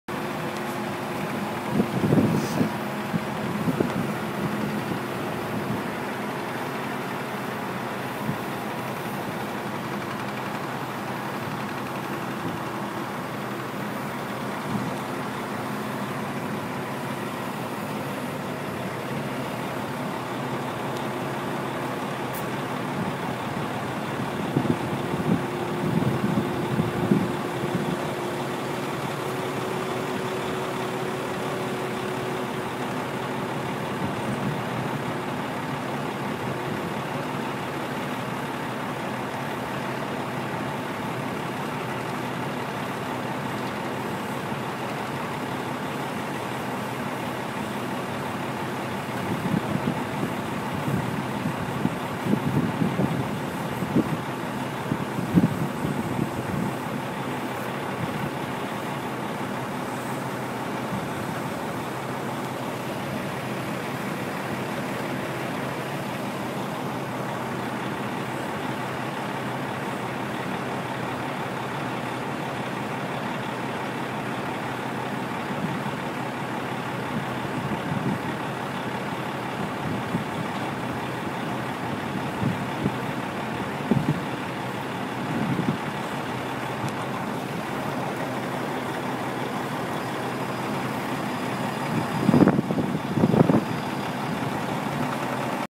Setting a septic tank with crane
We needed to get concrete tanks into the back yard and this was the only way to do it.